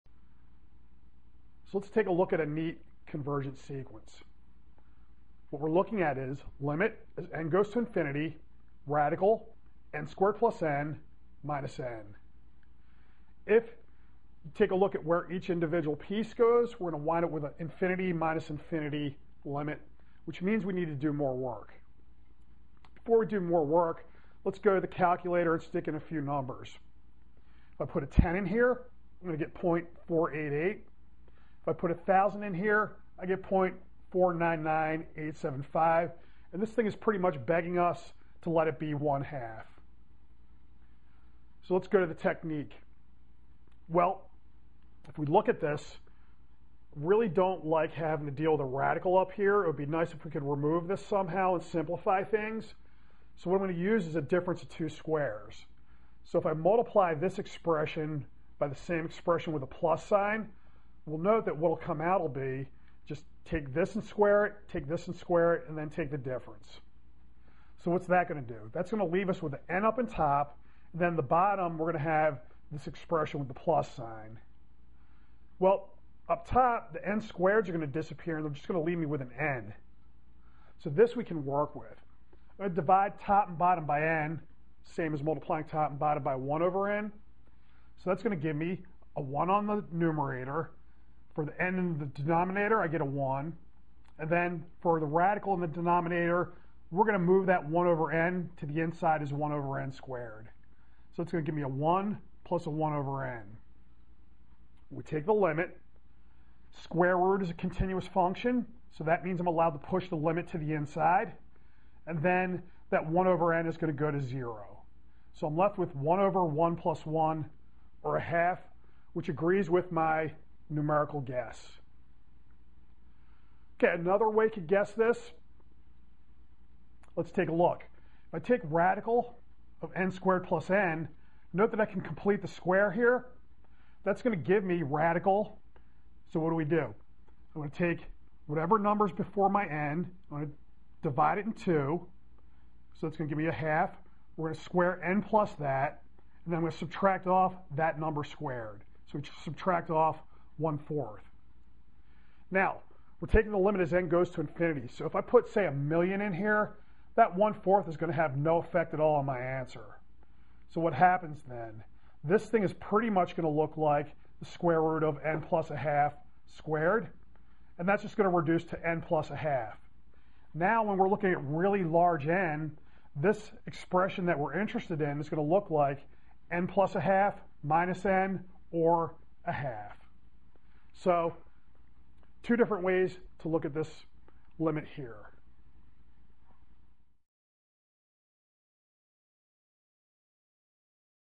Sequences 3 - Limit of sqrt(n^2 + n) - n
Calculus:   We calculate the limit  as n goes to infty of   sqrt(n^2 + n) - n using two methods.

For more videos like this one, please visit the Calculus playlists at this channel.